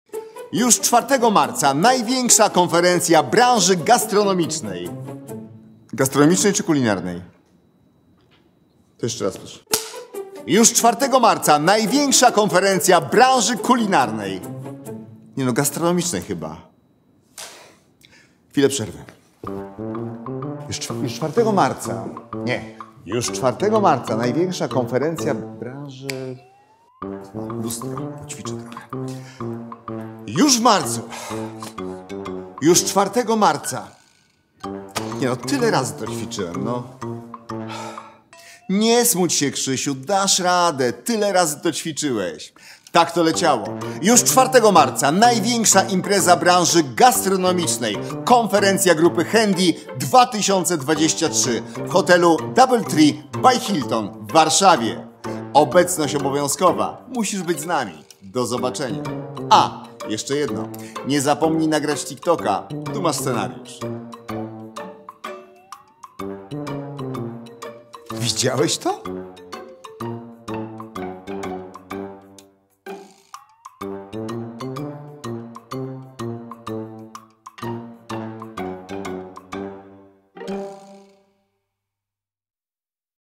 Krzysztof Ibisz - Zapowiedź Konferencji Hendi, Fine Dine, Grafen - największa impreza gastronomiczna
Razem z Krzysztofem Ibiszem mamy dla Was wspaniałą wiadomość!
Po 4 latach wracamy z największą imprezą branży gastronomicznej w Polsce.

Już 4-go marca w Warszawie odbędzie się Konferencja naszej Grupy, firm: Hendi, Finedine, Grafen, w trakcie której opowiemy Wam o planach na najbliższą przyszłość.
Uhonorujemy na niej także najlepszych dystrybutorów z całej Europy!

Konferencję uświetni występ wyjątkowego gościa... ale o tym dowiecie się wkrótce 😉

finedine.pl